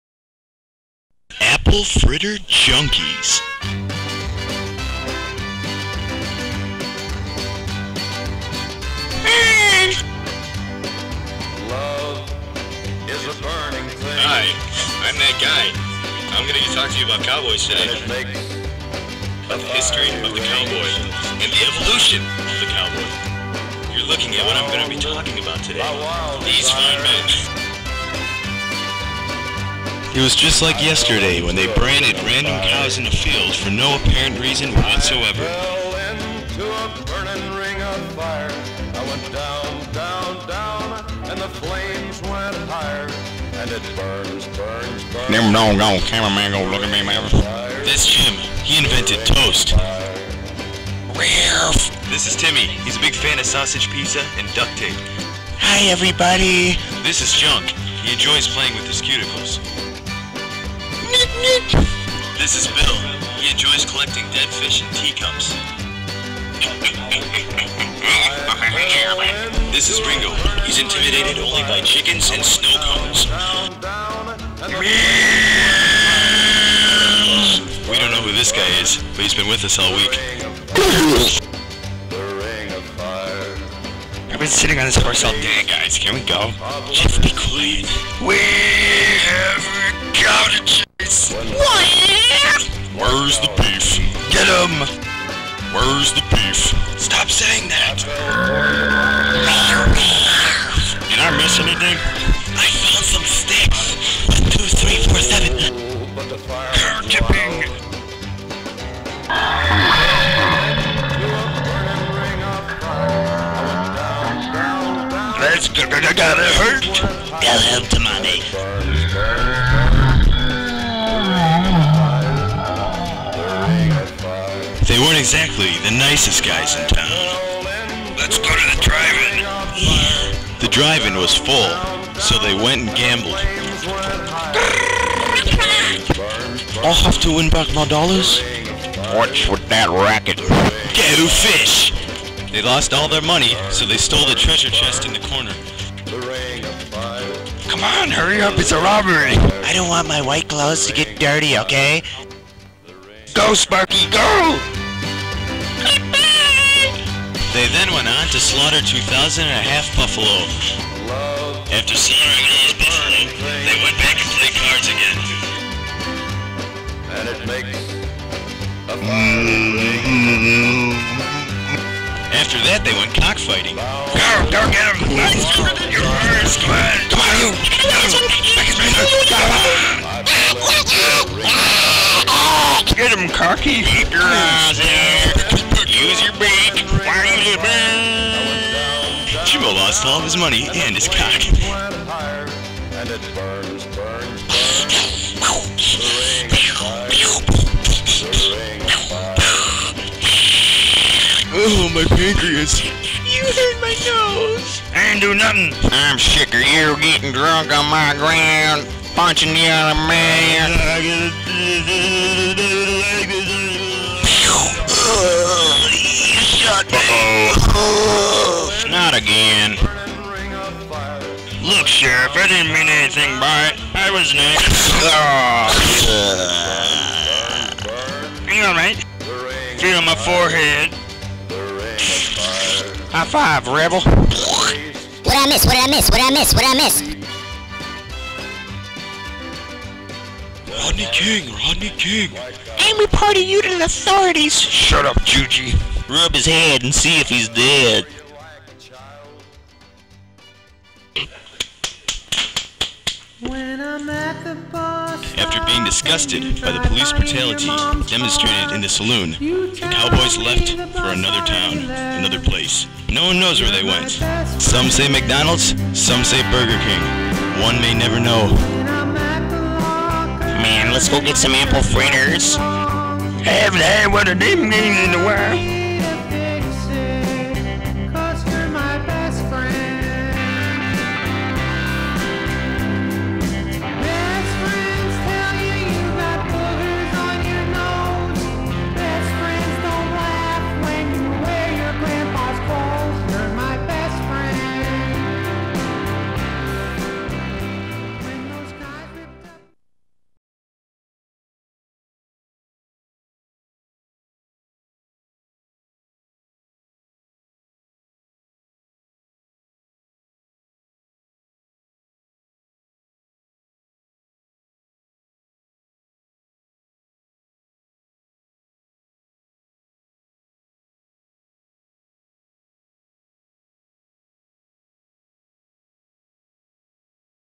Apple Fritter Junkies
Won the best comedy at my high school.  The introduction to my world of DUBS sandwiched bewteen a bunch of unforseen very lo fi classic DUBS.  This was my senior year when I won both best drama and best comedy based on editing old 16mm footage the school had